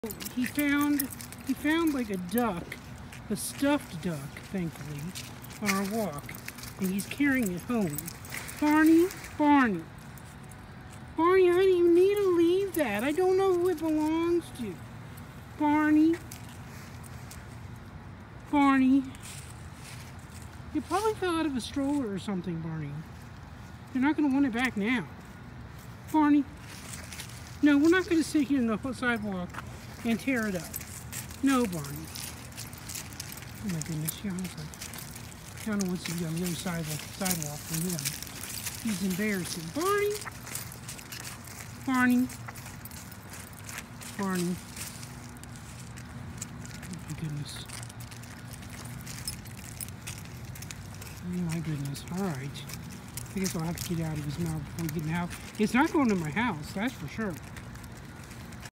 Basset Hound or Duck Hunter!
Barney found a stuffed duck on our walk and had a mind to bring it home... I did drop it before we got home. Too much work, I guess.